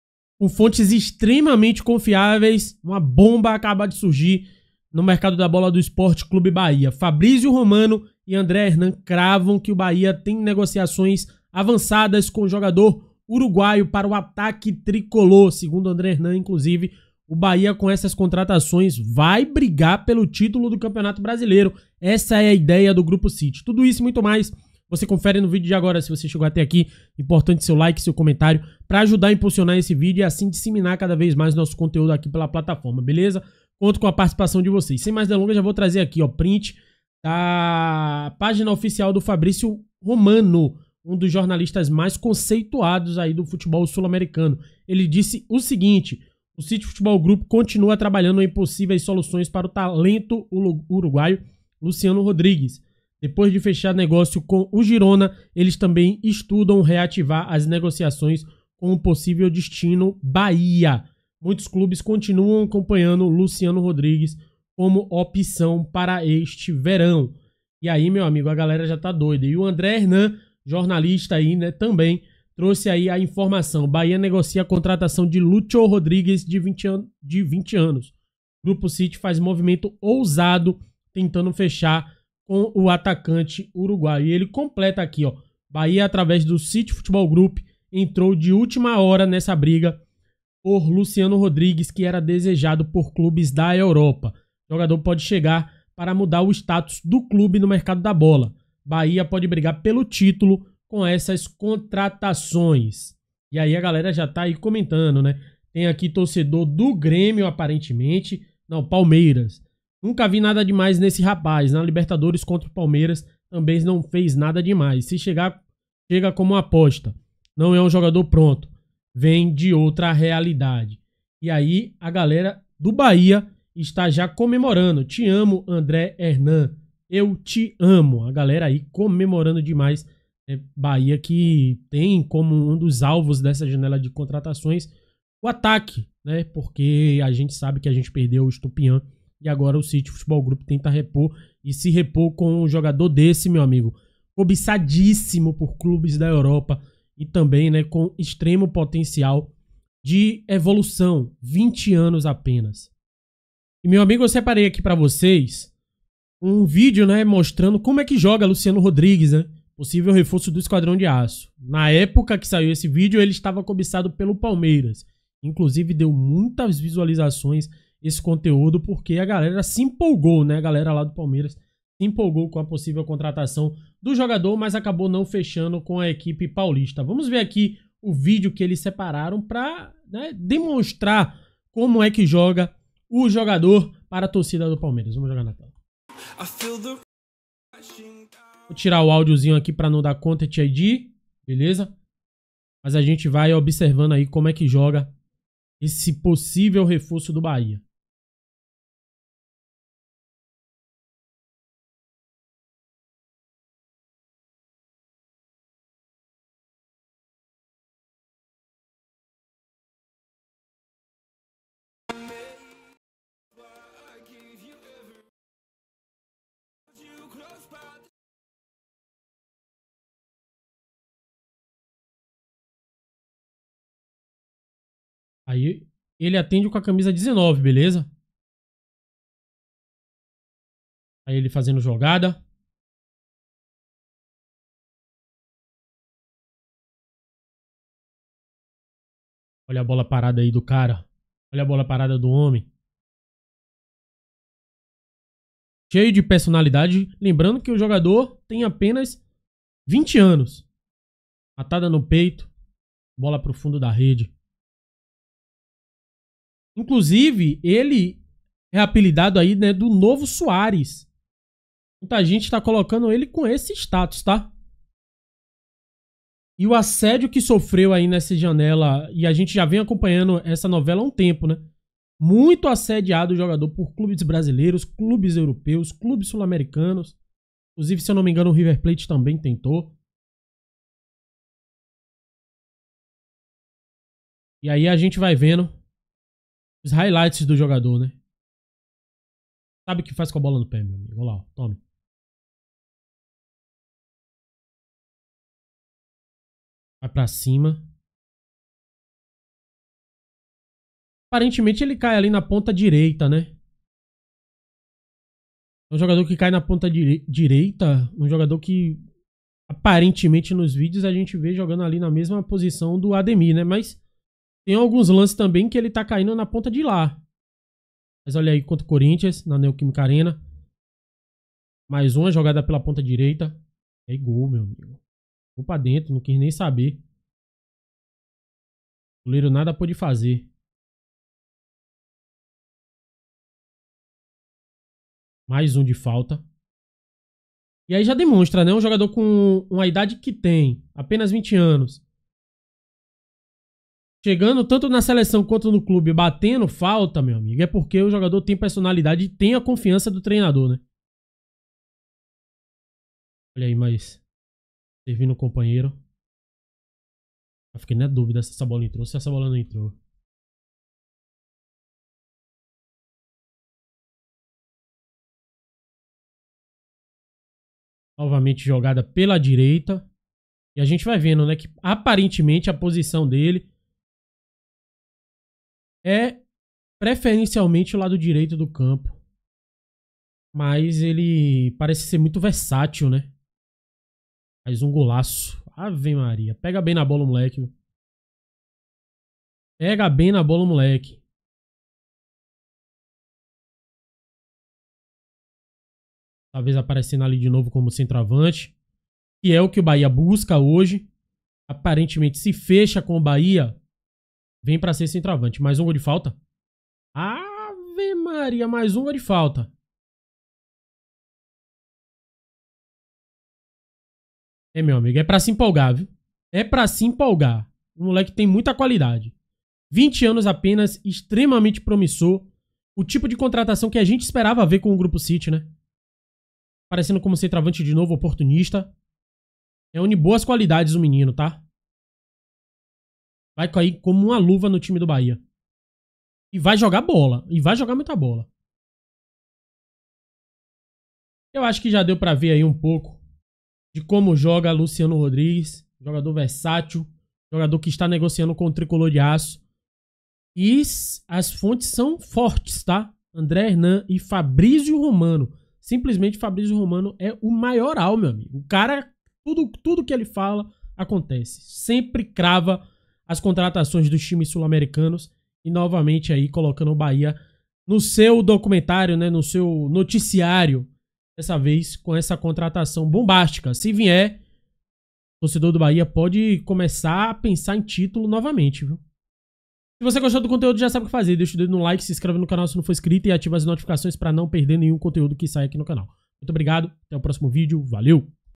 ABSURDO O QUE O BAHIA TA FAZENDO 😯 BAHIA PASSA POR CIMA DE PALMEIRAS E GIRONA E DEVE FECHAR NOVO 9
Seja Bem Vindo ao Tricolor ON.

Acompanhe a gente no Twitter